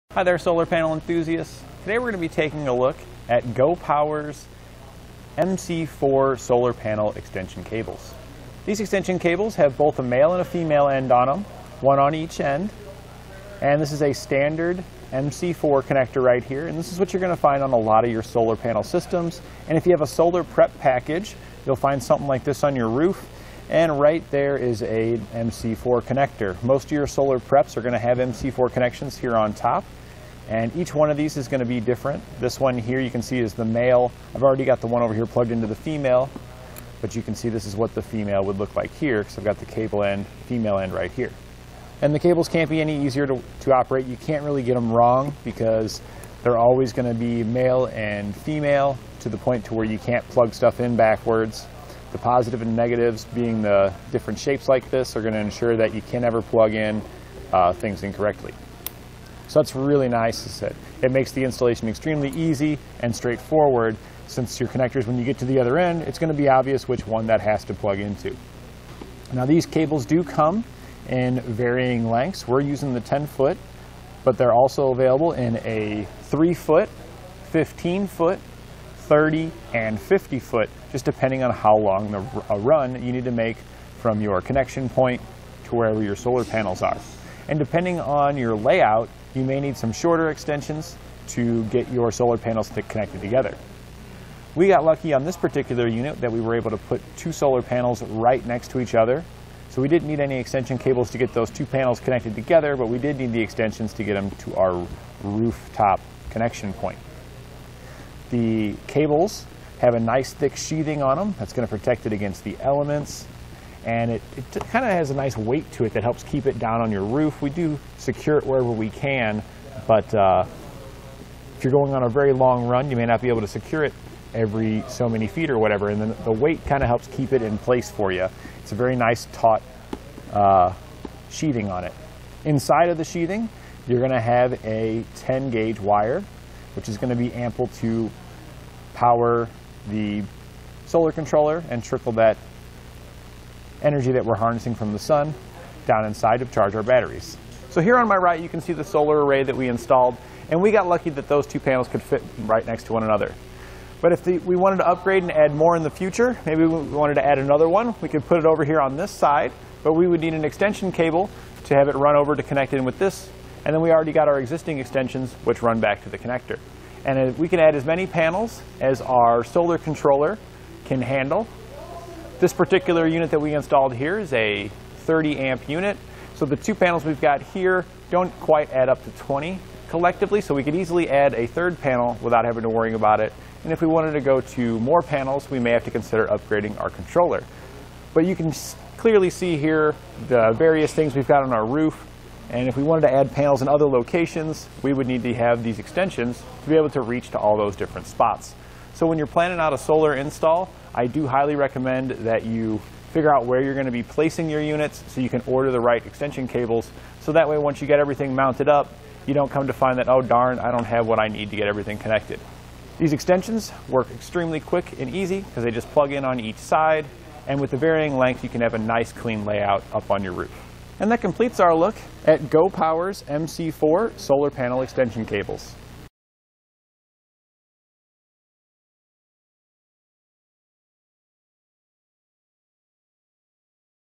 etrailer | Checking Out the Go Power MC4 Output Cable with Male and Female Connectors
Click for more info and reviews of this Go Power RV Solar Panels:

Hi there, solar panel enthusiasts. Today we're going to be taking a look at Go Power's MC4 solar panel extension cables. These extension cables have both a male and a female end on them, one on each end. And this is a standard MC4 connector right here. And this is what you're going to find on a lot of your solar panel systems. And if you have a solar prep package, you'll find something like this on your roof.

And right there is a MC4 connector. Most of your solar preps are going to have MC4 connections here on top. And each one of these is going to be different. This one here you can see is the male. I've already got the one over here plugged into the female.

But you can see this is what the female would look like here because we've got the cable end, female end, right here.And the cables can't be any easier to operate. You can't really get them wrong because they're always going to be male and female to the point to where you can't plug stuff in backwards. The positive and negatives being the different shapes like this are going to ensure that you can never plug in things incorrectly. So that's really nice, it makes the installation extremely easy and straightforward since your connectors, when you get to the other end, it's going to be obvious which one that has to plug into.Now, these cables do come in varying lengths. We're using the 10 foot, but they're also available in a three foot, 15 foot, 30, and 50 foot just depending on how long a run you need to make from your connection point to wherever your solar panels are.

And depending on your layout, you may need some shorter extensions to get your solar panels to connect it together. We got lucky on this particular unit that we we're able to put two solar panels right next to each other. So we didn't need any extension cables to get those two panels connected together, but we did need the extensions to get them to our rooftop connection point.The cables have a nice thick sheathing on them. That's going to protect it against the elements. And it kind of has a nice weight to it that helps keep it down on your roof.

We do secure it wherever we can, but if you're going on a very long run, you may not be able to secure it every so many feet or whatever. And then the weight kind of helps keep it in place for you. It's a very nice taut sheathing on it. Inside of the sheathing, you're going to have a 10 gauge wire, which is going to be ample to power the solar controller and triple that energy that we're harnessing from the sun down inside to charge our batteries.So here on my right, you can see the solar array that we installed. And we got lucky that those two panels could fit right next to one another. But if we wanted to upgrade and add more in the future, maybe we wanted to add another one, we could put it over here on this side, but we would need an extension cable to have it run over to connect in with this. And then we already got our existing extensions which run back to the connector. And we can add as many panels as our solar controller can handle. This particular unit that we installed here is a 30 amp unit. So the two panels we've got here don't quite add up to 20 collectively. So we could easily add a third panel without having to worry about it. And if we wanted to go to more panels, we may have to consider upgrading our controller.But you can clearly see here the various things we've got on our roof. And if we wanted to add panels in other locations, we would need to have these extensions to be able to reach to all those different spots. So when you're planning out a solar install, I do highly recommend that you figure out where you're going to be placing your units so you can order the right extension cables. So that way, once you get everything mounted up, you don't come to find that, oh darn, I don't have what I need to get everything connected. These extensions work extremely quick and easy because they just plug in on each side. And with the varying length, you can have a nice clean layout up on your roof. And that completes our look at Go Power's MC4 solar panel extension cables.